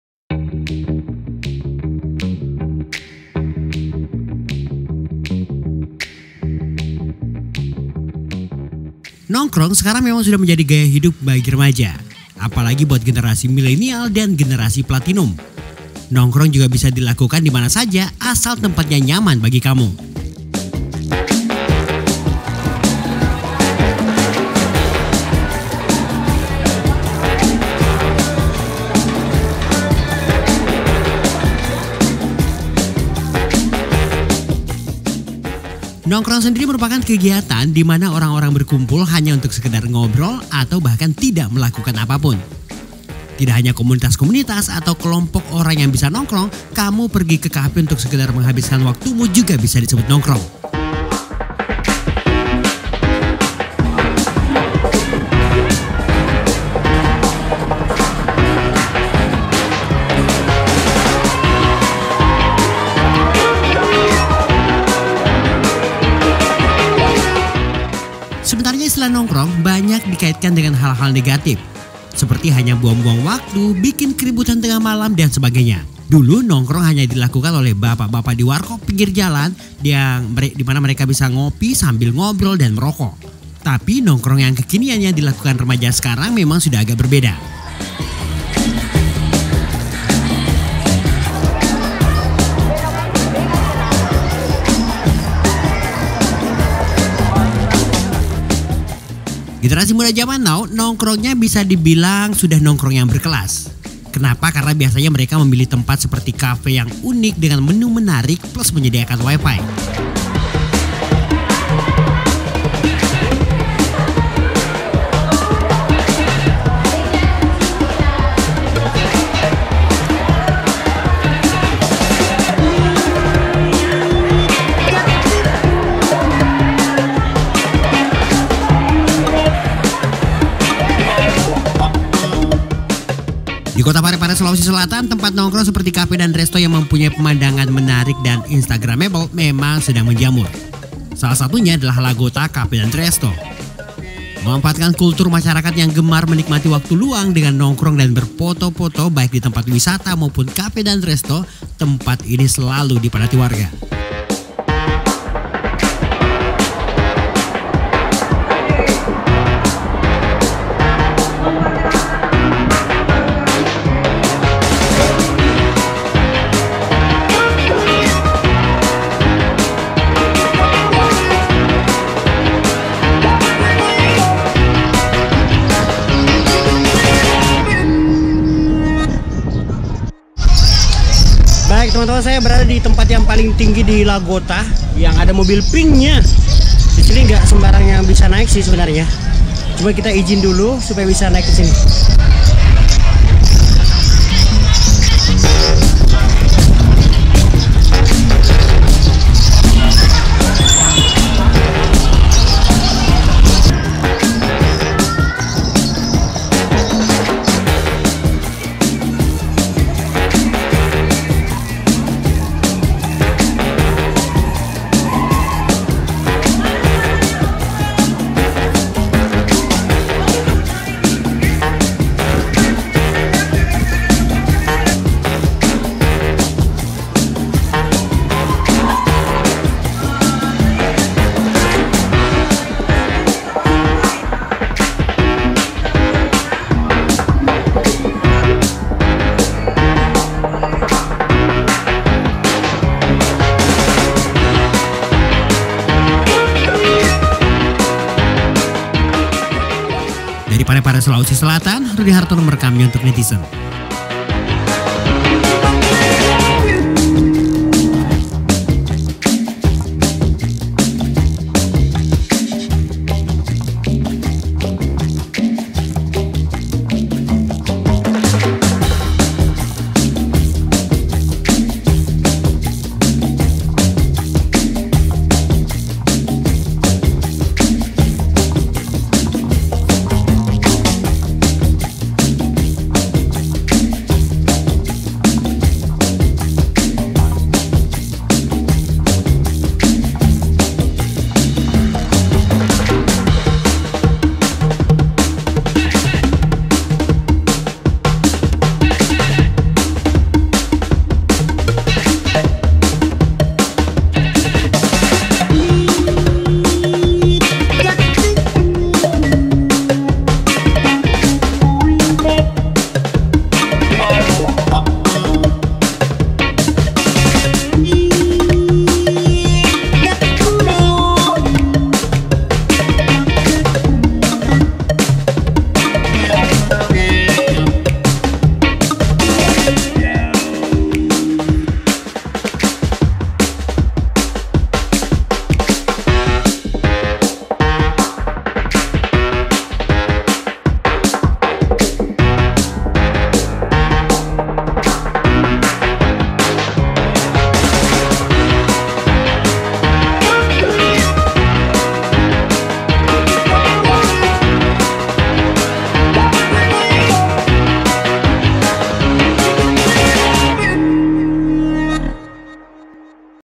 BUDAYA NONGKRONG MILLENIAL DAN PLATINUM YANG SEMAKIN KAFE DAN RESTO MENJAMUR
Nongkrong sekarang udah jadi gaya hidup nih generasi muda apalagi buat generasi milenial dan generasi platinum. Nongkrong juga bisa dilakukan dimana saja asal tempatnya nyaman bagi kamu. Nongkrong sendiri merupakan kegiatan dimana orang-orang berkumpul hanya untuk sekedar ngobrol, atau bahkan tidak melakukan apapun. Tidak hanya komunitas-komunitas atau sekelompok orang yang bisa nongkrong, kamu pergi ke kafe untuk sekedar menghabiskan waktumu juga bisa disebut nongkrong.

Sebenarnya, istilah nongkrong banyak dikaitkan dengan hal-hal negatif, seperti hanya buang-buang waktu, bikin keributan tengah malam dan sebagainya. Dulu, nongkrong hanya dilakukan oleh bapak-bapak di warkop pinggir jalan, mereka ngopi sambil ngobrol dan merokok. Tapi nongkrong yang kekinian yang dilakukan generasi muda sudah agak berbeda.

generasi muda jaman now, nongkrongnya bisa dibilang udah nongkrong yang berkelas. Kenapa? Karena biasanya mereka memilih tempat seperti kafe yang unik dengan menu menarik plus menyediakan Wifi.

Di kota Parepare sulawesi selatan tempat nongkrong seperti kafe dan resto yang mempunya pemandangan menarik dan instagramable memang sedang menjamur. salah satunya adalah lagota kafe dan resto. Memanfaatkan kulturt ,masyarakat yang gemar menikmati waktu luang dengan nongkrong dan berfoto foto baik di tempat wisata maupun kafe dan resto.